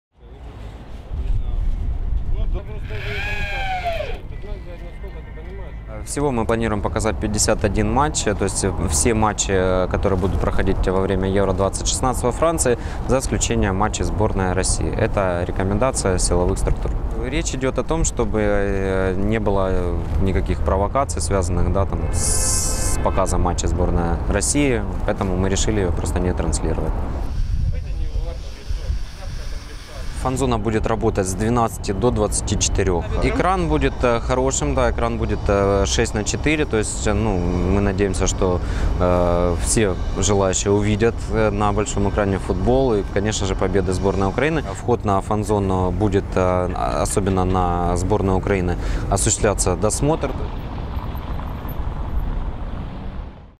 Матчи с Россией на Евро-2016 не покажут в фан-зоне Харькова
В харьковской фан-зоне Евро-2016, которая откроется на площади Свободы, не планируют показывать матчи с участием сборной России. Такое решение приняли в горсовете по рекомендации силовых структур.